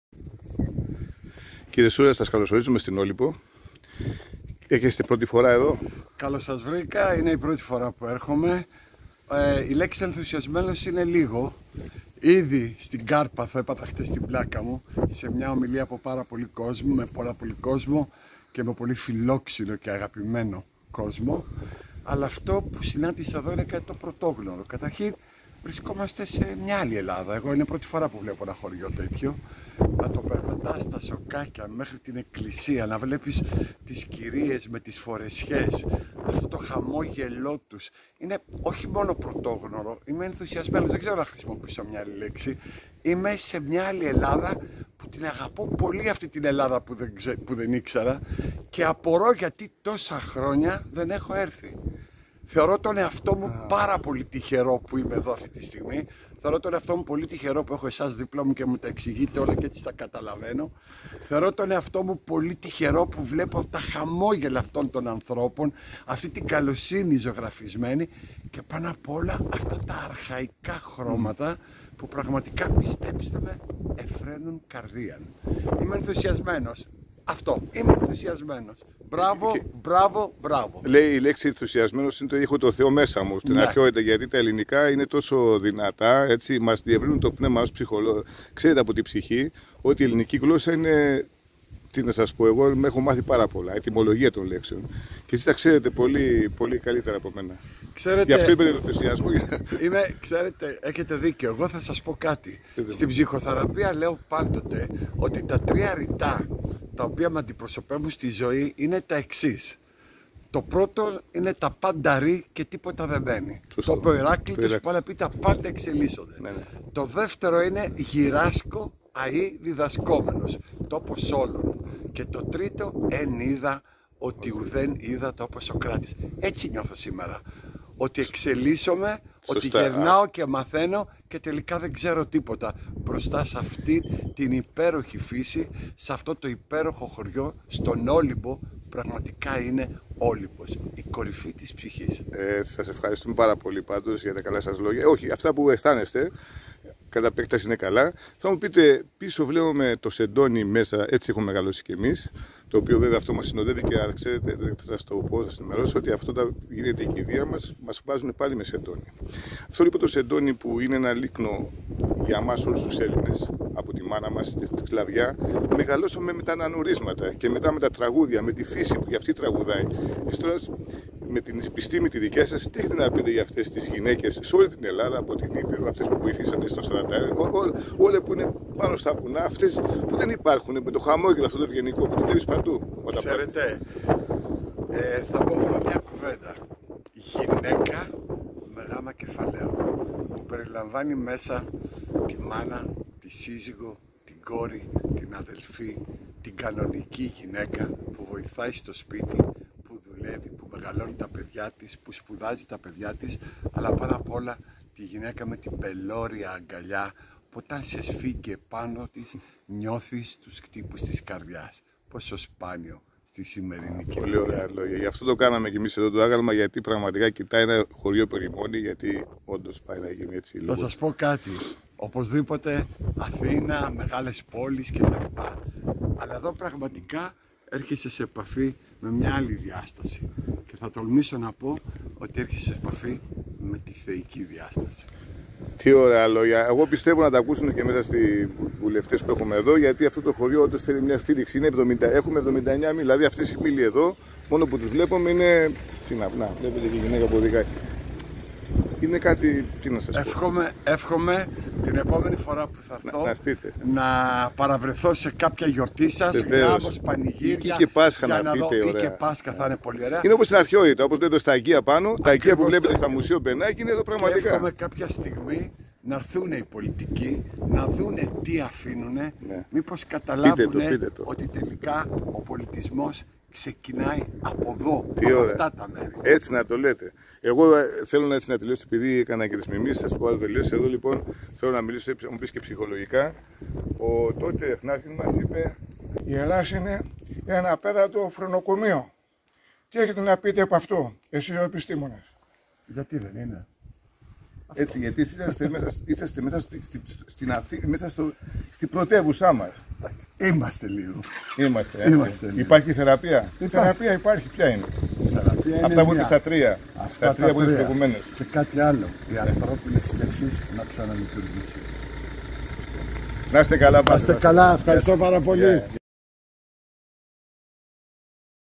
ΟΛΥΜΠΟΣ ΚΑΡΠΑΘΟΥ Ο διακεκριμένος ψυχίατρος Δημήτρης Σούρας - Μουσείο Βασίλη Χατζηβασίλη
ΟΛΥΜΠΟΣ ΚΑΡΠΑΘΟΥ
Ο διακεκριμένος επιστήμονας , Δημήτρης Σούρας   Διευθυντής της Ψυχιατρικής Κλινικής του Νοσοκομείου "Metropolitan" και Υπεύθυνος Ψυχίατρος του Ωνασείου Καρδιοχειρουργικού Κέντρου σε θέματα προ και μεταμοσχευτικού ελέγχου.
Συναντηθήκαμε εντελώς τυχαία μετά το πέρας της εκδήλωσης για την  7 Μαρτίου που έγινε σήμερα Κυριακή, έτσι μας είπε  λίγα λόγια που πραγματικά έχουν βάθος για την Όλυμπο και την εντύπωση του γενικα για την Κάρπαθο.
Να  σημειώσω ότι βρίσκεται εδώ ως κεντρικός ομιλητής σε εκδήλωση που έγινε χθες ,
για την σχολική βία και των εθισμό από ηλεκτρονικά παιχνίδια, με κεντρικό .
Ιδέα και οργάνωση από τον Σύλλογο του Γυμνασίου Πηγαδίων Καρπάθου και της καθηγήτρια κας Σταματοπούλου.